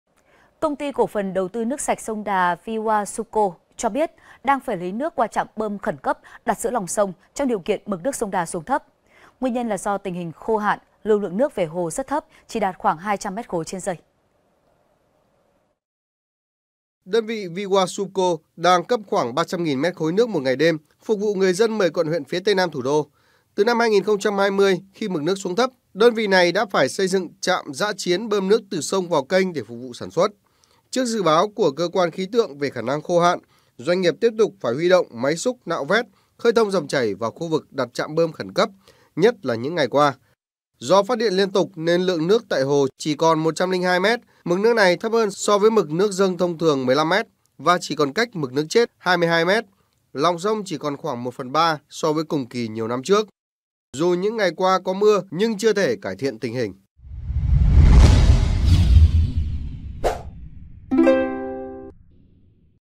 Lắp trạm bơm khẩn cấp vì sông Đà khô cạn | VTC14
VTC14 | LẮP TRẠM BƠM KHẨN CẤP VÌ SÔNG ĐÀ KHÔ CẠN
Công ty cổ phần Đầu tư nước sạch Sông Đà (Viwasupco) cho biết, đang phải lấy nước qua trạm bơm khẩn cấp đặt giữa lòng sông trong điều kiện mực nước sông Đà xuống thấp. Nguyên nhân là do tình hình khô hạn, lưu lượng nước về hồ rất thấp chỉ đạt khoảng 200m3/s.